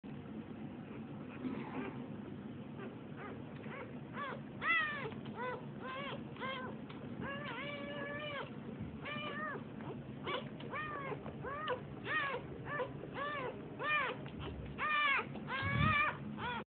American Molossus pups just born Old World Chief and Old World Onyx
Pick up my book Dog Breeding secrets by Marcus E Curtis and Check out the American Molossus Kennel club